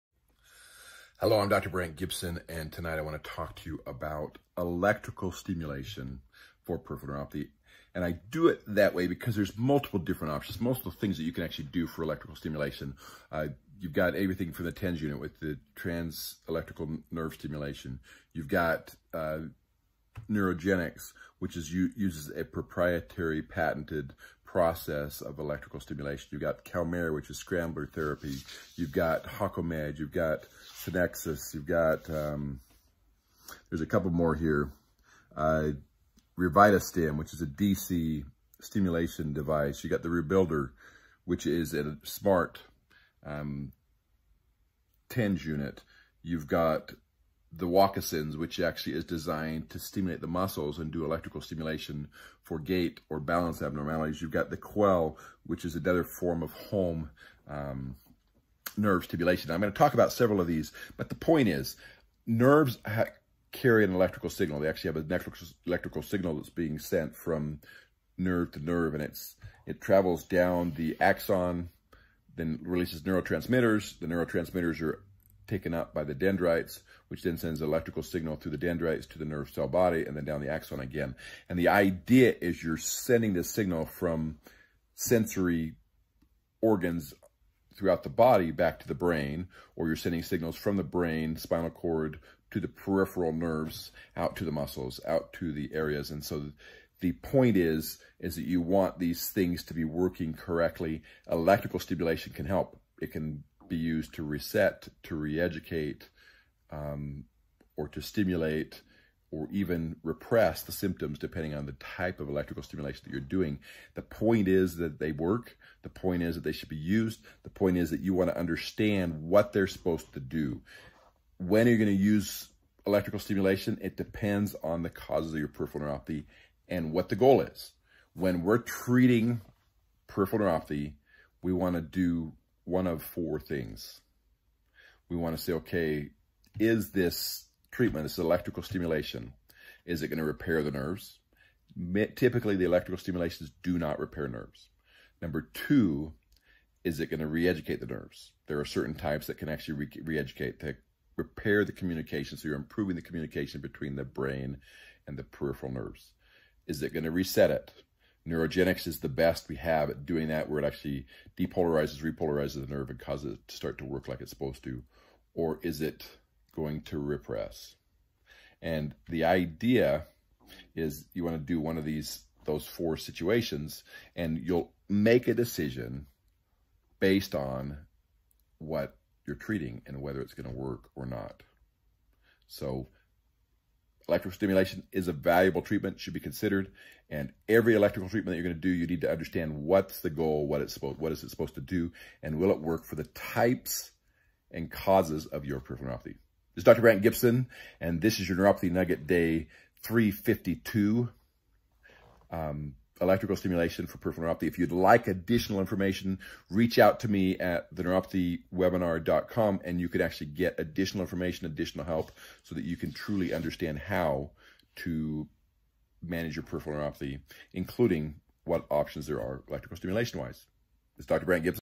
Electrical Stimulation For Peripheral Neuropathy
Neuropathy Nugget Day 352 - Electrical stimulation is a valuable treatment option for Peripheral Neueopathy. Dr Brandt R Gibson discisses this. Neuropathy NeuropathyNation To learn more visit TheNeuropathyWebinar.com.